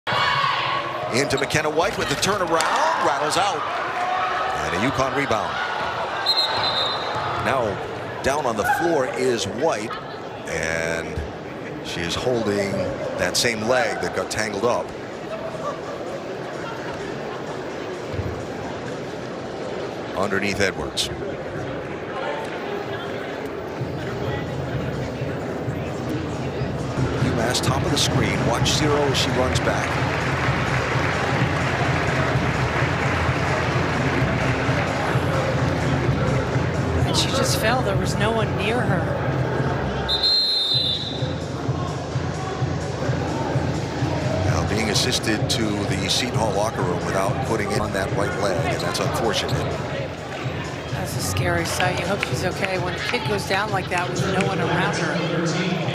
🤕 NON-CONTACT Injury, Senior Makennah White Helped Into Locker Room | #9 UConn Huskies vs Seton Hall
Code: aav17854